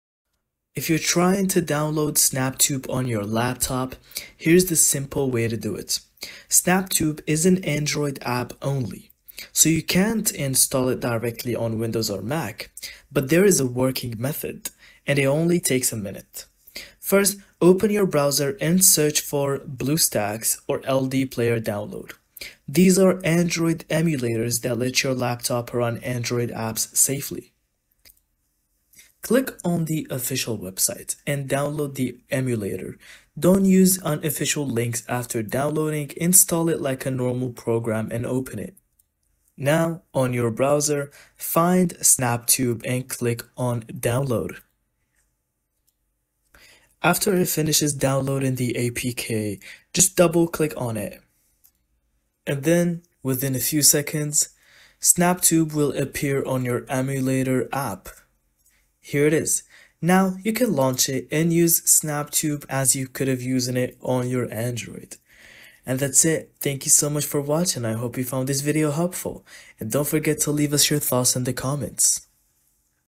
CAN YOU DOWNLOAD SNAPTUBE ON LAPTOP & PC (2026 GUIDE)
Learn how to safely download Snaptube on your laptop using the latest working methods in 2025. This guide covers how to install Snaptube on Windows or Mac using Android emulators, what websites to avoid, and the safest alternatives for video downloading on a laptop. Perfect for beginners who want a smooth, fast, and secure installation process.